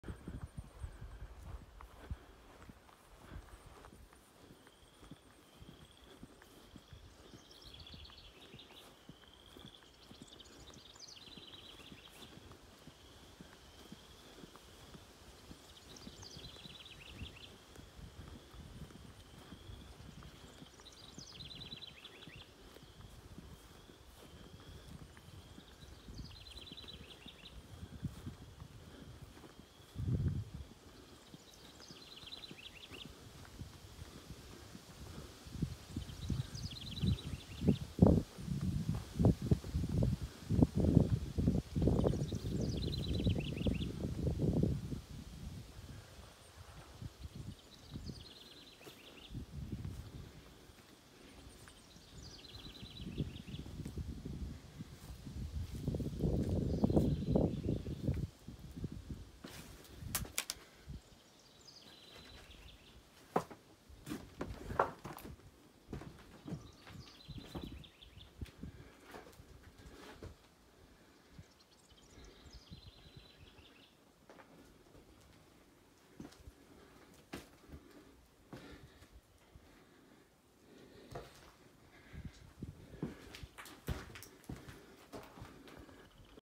Perfect property for Moonshining, poaching , and hunting cheerleaders
A real fixer upper near the lake and deep in the woods. There’s no neighbours for miles so you can go ahead and make all the noise you want. Great for a weekend getaway for when you really want to let loose. Moonshining, meth making , hunting people .. let the imagination run wild.. just kidding .. maybe